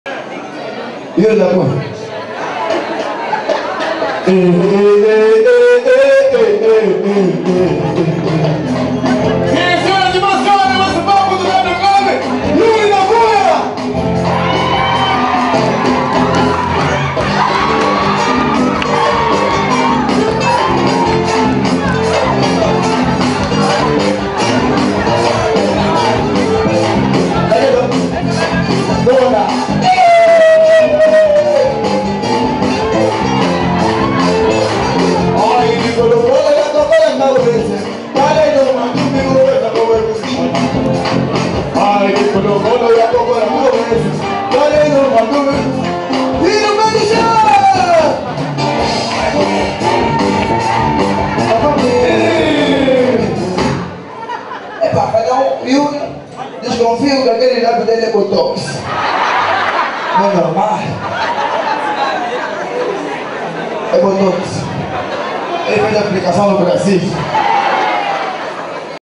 Calado Show ESTILO Yuri da cunha
comediante / Homorista CALADO SHOW na discoteca W (antigo chiwawa) em angola no mes de Junho/ Julho de 2010.

Imagens de Ivandro Pereira,
Luzes     de  Ivandro Pereira,
Som        de Ivandro Pereira,
Edicao    de Ivandro Pereira